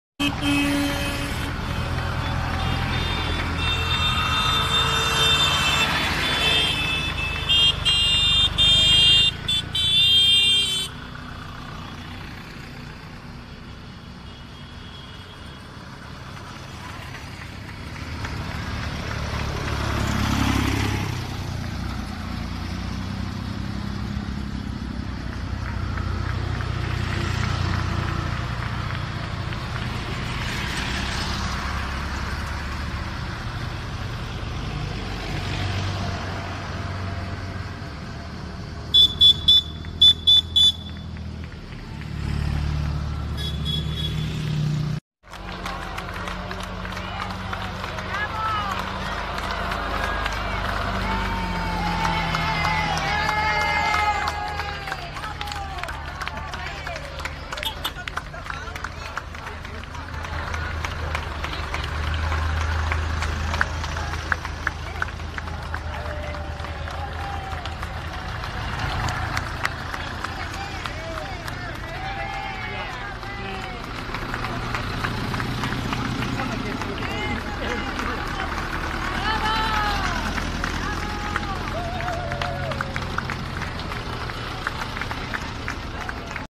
8-8-18 Mes de 200 tractors a la presó de Mas d'Enric.(by NaciòDigital/Tgn)
Més de 200 tractors han participat en l'acte reivindicatiu pels presos polítics a la presó de Mas d'Enric, on hi ha l'expresidenta del Parlament, Carme Forcadell. La crida, feta per Pagesos per la República, ha rebut una bona resposta, ja que centenars de persones s'han aplegat a l'aparcament del centre penitenciari per mostrar el seu suport a Carme Forcadell i agrair als pagesos la seva iniciativa.